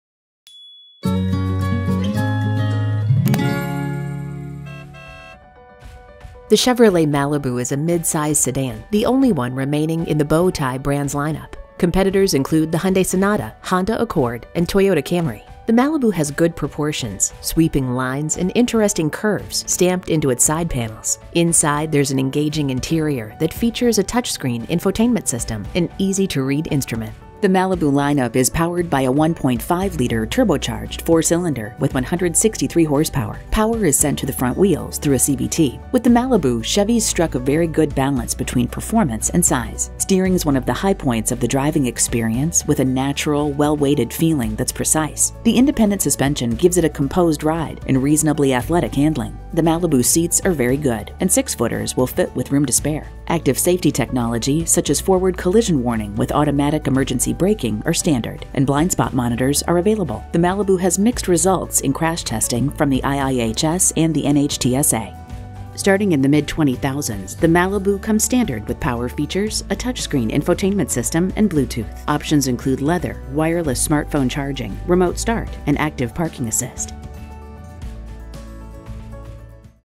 Chevrolet Malibu overview
The Chevrolet Malibu is a midsize sedan, the only one remaining in the bowtie brand’s lineup.